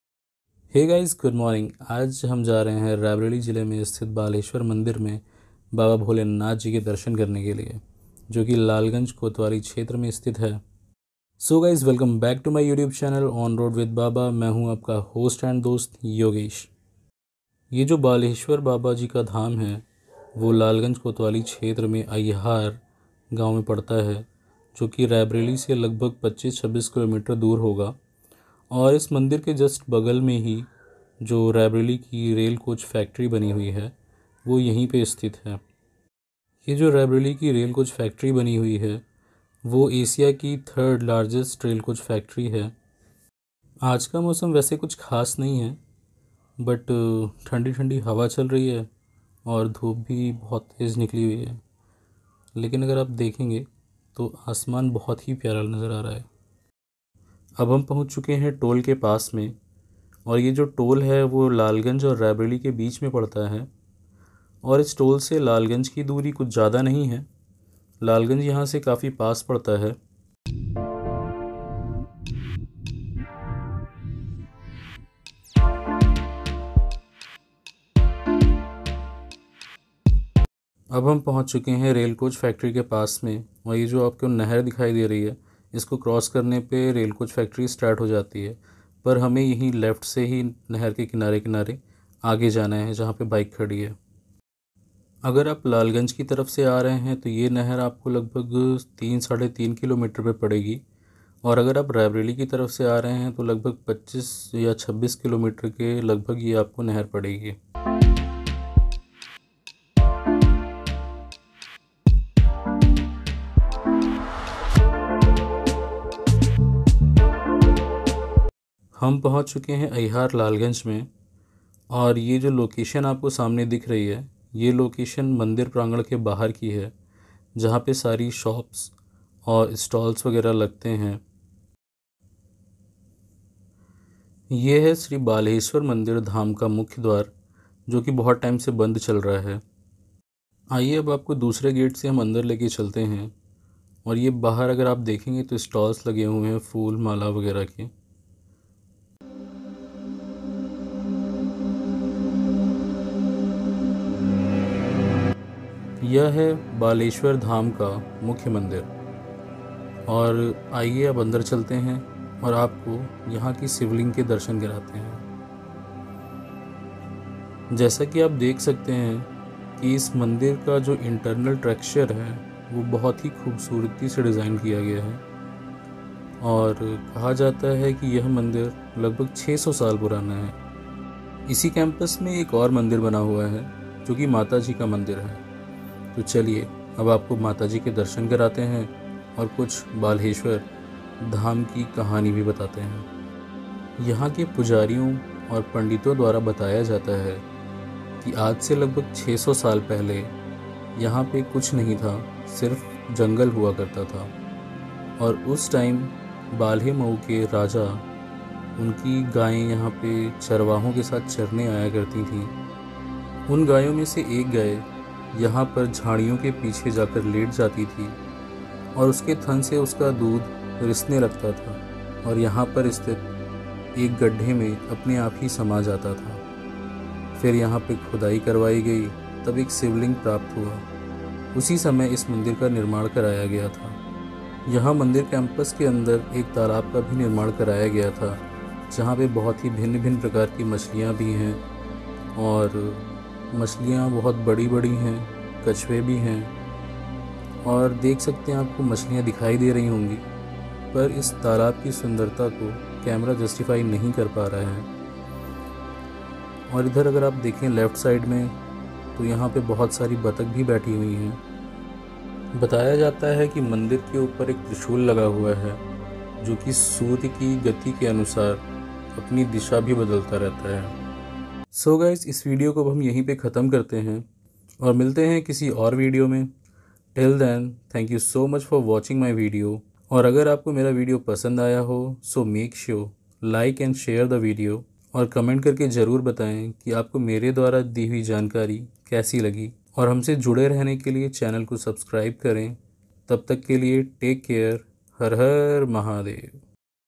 श्री बाल्हेश्वर बाबा मन्दिर धाम लालगंज रायबरेली | Balheshwar Mandir Lalganj Aihar Raebareli
श्री बाल्हेश्वर बाबा मन्दिर धाम लालगंज रायबरेली | Balheshwar Mandir Lalganj Aihar Raebareli U.P

                                  Is Video Me Main Aap Logo Ko Shree Balheshwar Mandir Lalganj Ke Darshan Karaunga Aur Is Mandir Se Judi Hui Manyata Aur Kahani Bataunga

Using Gears
My Phone

My Tripod

My Mic

Action camera

Action camera mic